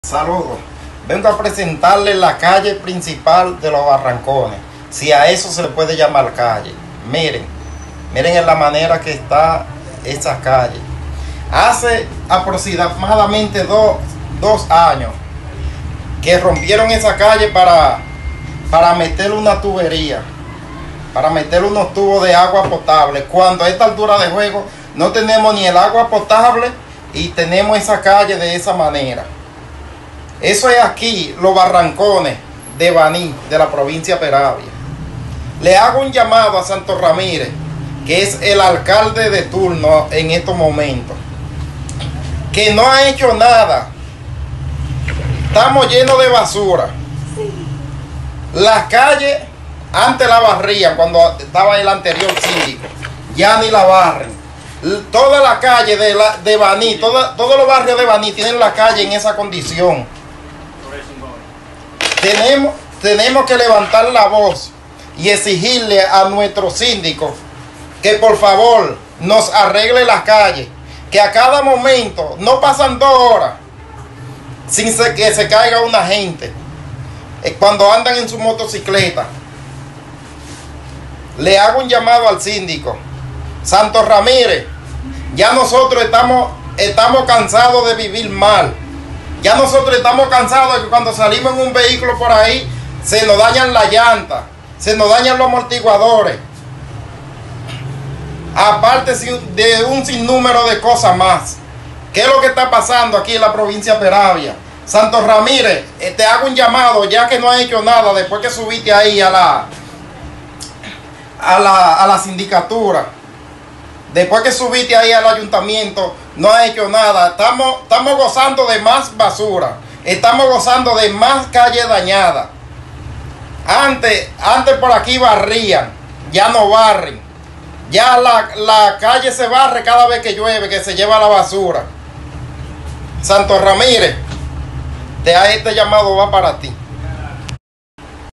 Moradores del sector Los Barrancones Bani Provincia peravia reclaman arreglo de sus calles.
Se quejan por las malas condiciones de sus calles y la falta de agua potable 💦 .